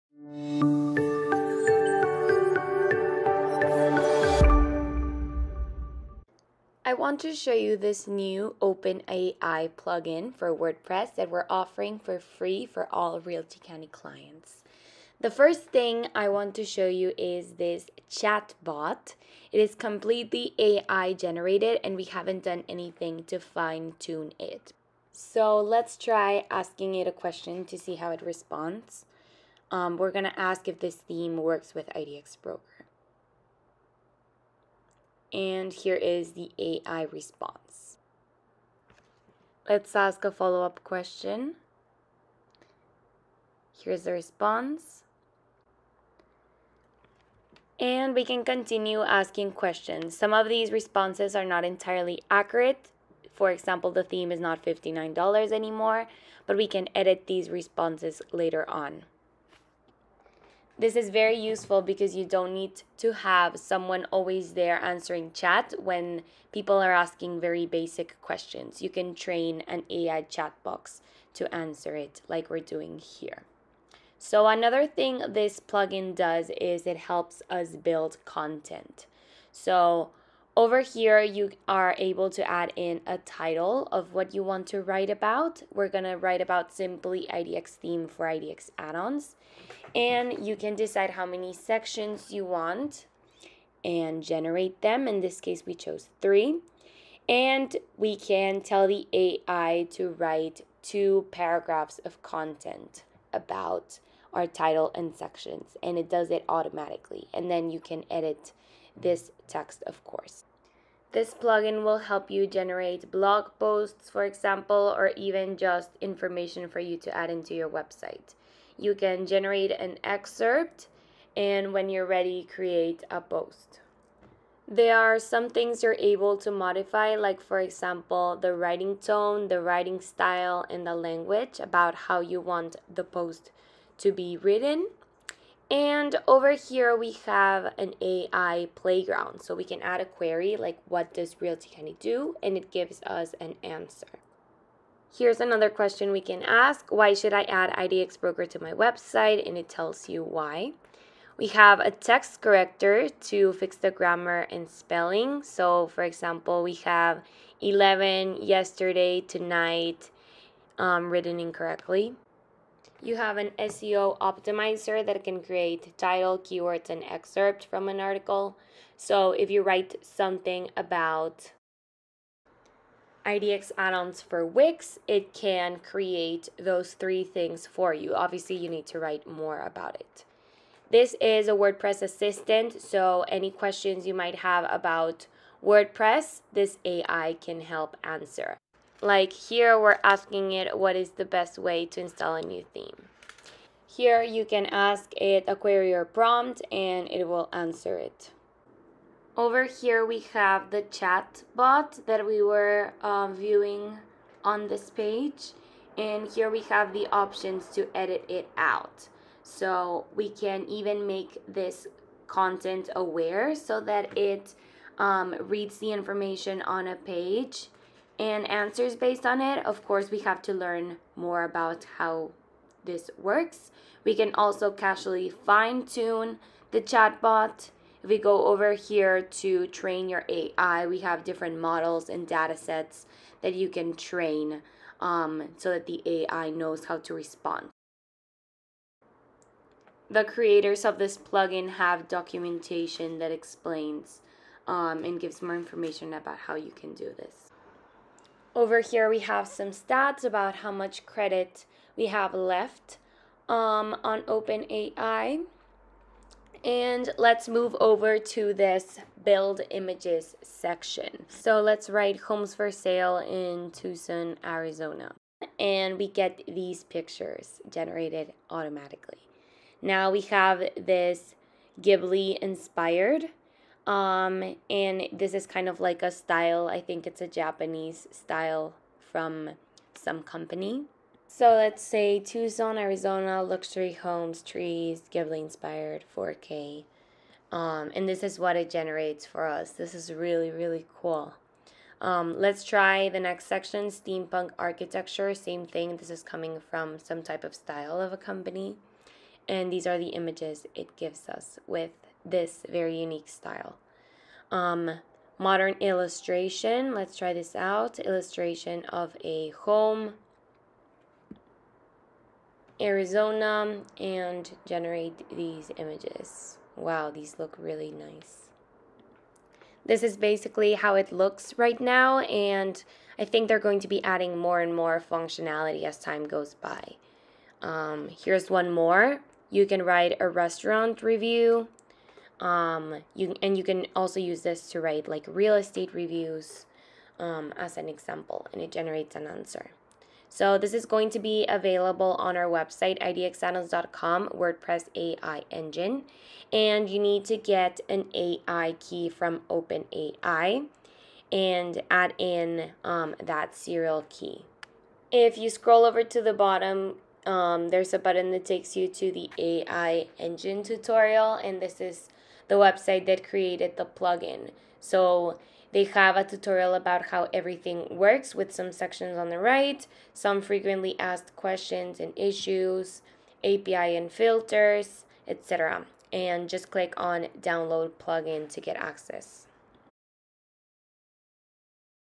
Generate content and add a chat widget using AI Plugin for Wordpress with IDXBroker and RealtyCandy
RealtyCandy is offering this new AI Wordpress plugin for your real estate website. This is completely free for IDXAddons clients whether that be Lite or Premium for Free. We have a download link on IDXAddons.com for you to get access to it and use AI to add a chat widget to your real estate site, generate images, blog posts, answer questions, and more. More features are being added to the plugin periodically so keep checking back for new updates!

to send an email to IDXBroker and make the change